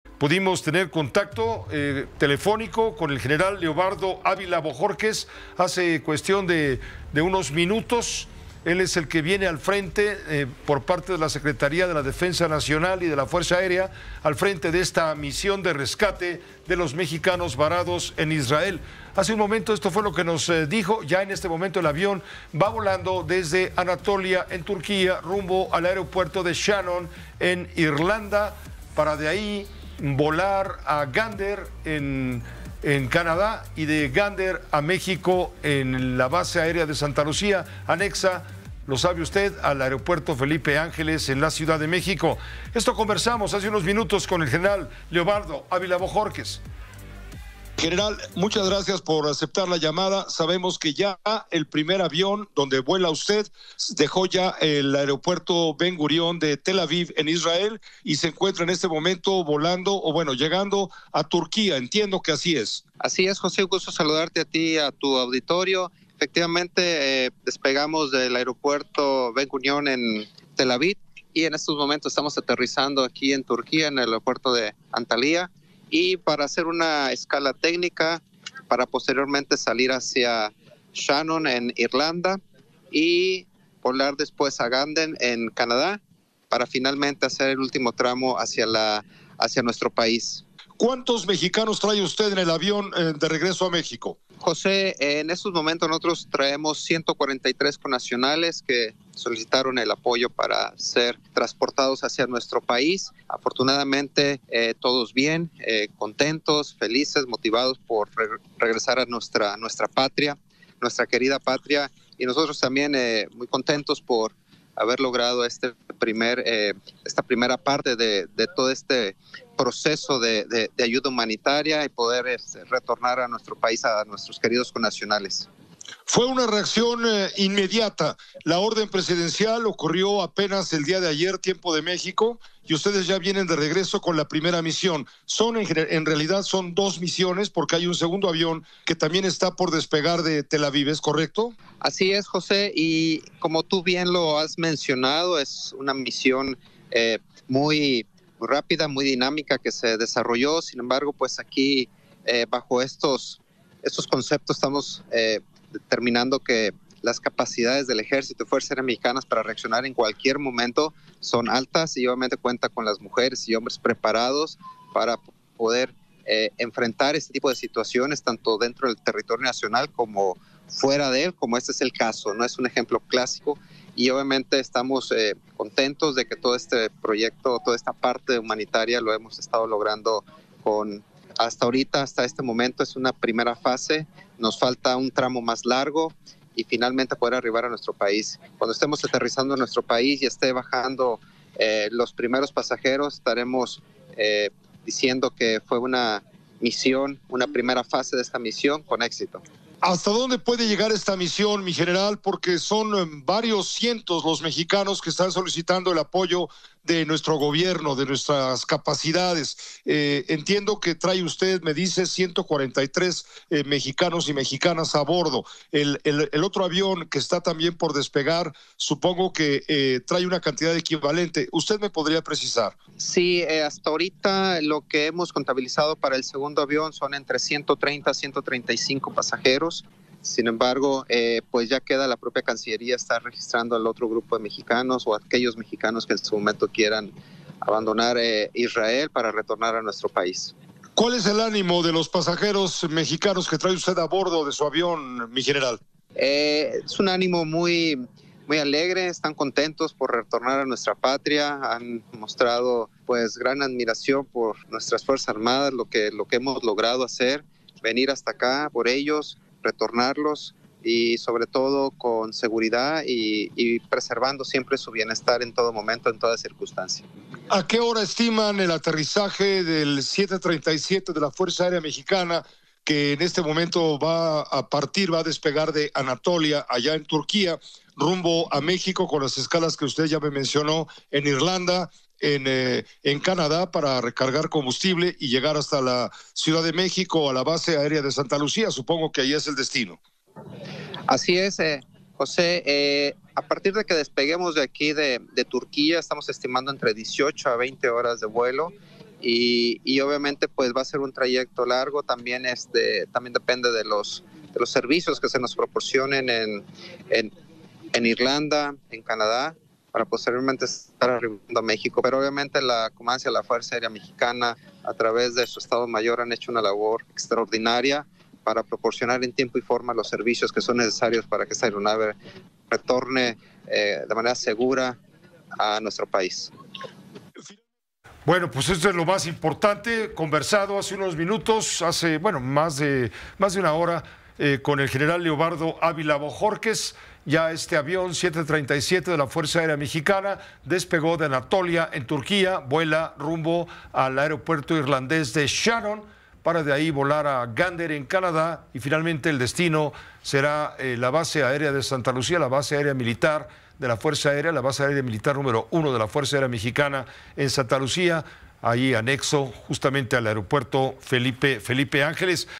Salió de Israel primer avión de la Fuerza Aérea Mexicana con 143 connacionales
El primer avión de la Fuerza Aérea Mexicana salió rumbo a México desde Israel con 143 connacionales que huyen de la guerra que mantiene dicho país con el grupo Hamas, asentado en Palestina, y que durante el fin de semana realizo actos considerados terroristas que han dejado más de 1 mil 500 personas muertas.

La misión, que lleva 22 horas de vuelo y ha hecho escalas en Canadá, Irlanda del Norte y Turquía, embarcó hace unos momentos al último mexicano que podrá salir de Israel de los cerca de 1 mil que buscan salir del país asiático.

Grupo Fórmula